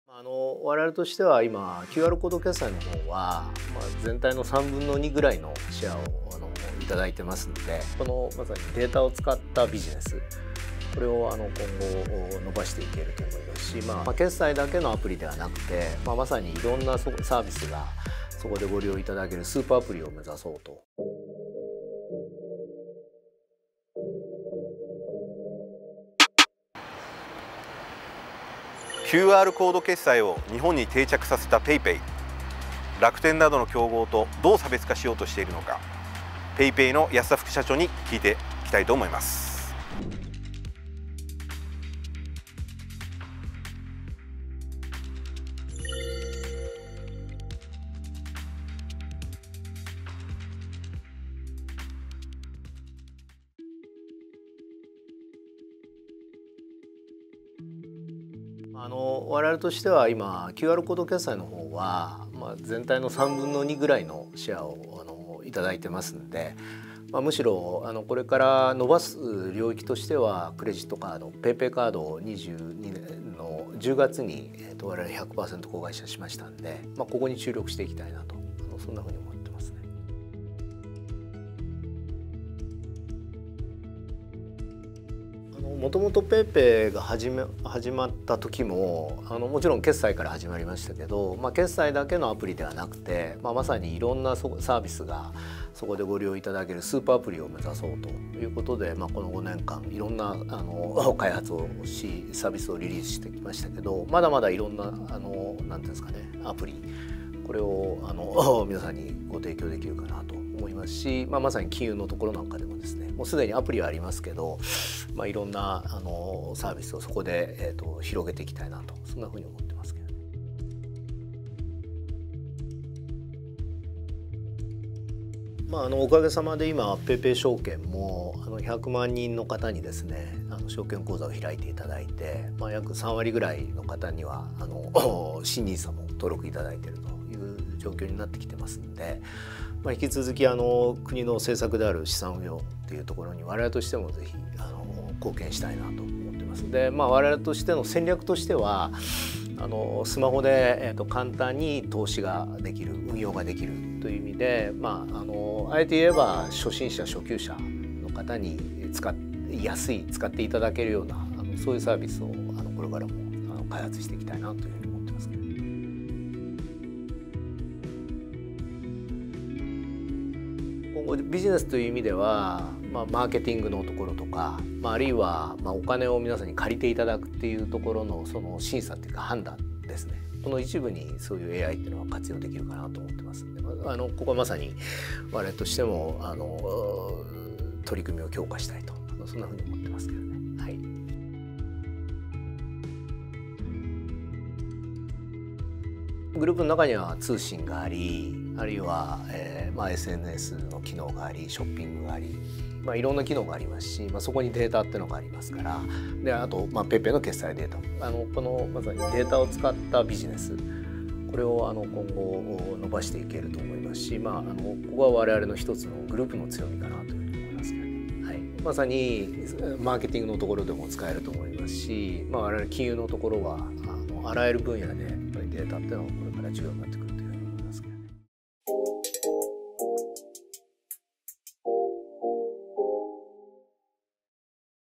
PayPay安田正道副社長インタビュー
PayPayの安田正道副社長にインタビューしました。以下のURLから記事をお読みいただけます。

●PayPay副社長「金融サービスのユーザー増やしキャッシュレスのトップに」